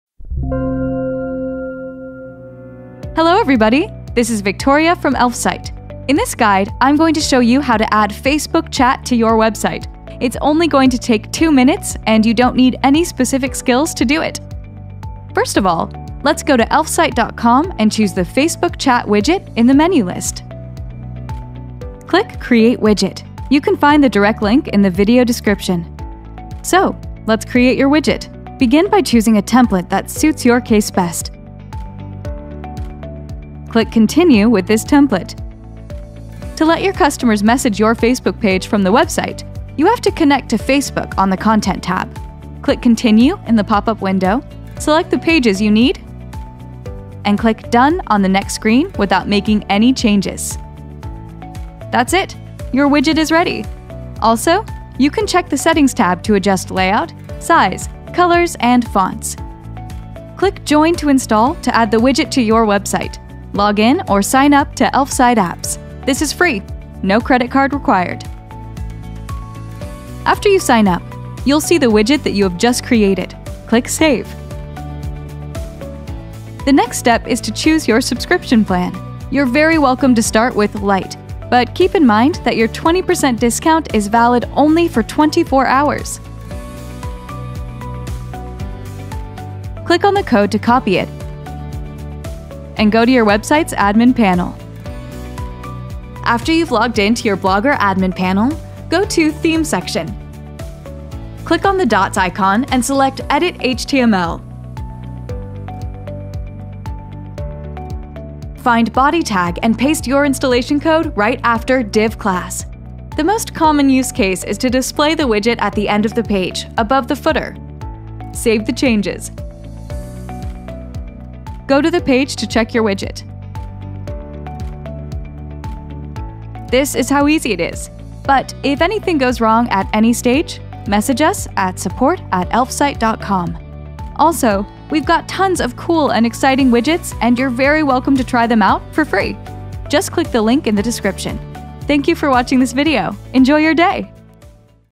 How to Add Facebook Messenger Widget to Blogger
Add Blogger Facebook Chat widget to site in less than a couple of minutes -

Offer the audience the chance to message you on Facebook right from the website. Boost the quality of customer service, improve sales, increase the number of leads.

Characteristics:

• A large set of bubble icons to choose from.
• Option to edit a chat name.
• Selection of Five Reply time variants in the chat window.
• Chat header with caption, and page picture name.
• Editable bubble text.

How to embed Facebook Messenger on Blogger website?

To embed our plugin, follow the actions from the detailed guideline:

1. Shape and adjust the widget;
2. Copy a unique code from the notification;
3. Place this code on the website.

Check how to add Blogger Messenger plugin by clicking this link -

Best Facebook Chat widget integration examples:

Example 1: General - the most common floating Facebook chat bubble for a website in distinguished colors of the platform.
Example 2: Support - photo of the customer support manager is attached to the header, as well as dots animation imitating the manager typing a message.
Example 3: Sale - this example is perfect to tell customers about the upcoming deal from the face of your sales agent.

Here’s how easy it is to add a Messenger plugin to Blogger! If you need to learn more about widget templates, benefits, and adjustments, you are free to write to our competent Support experts. We’ll be happy to respond to all your questions!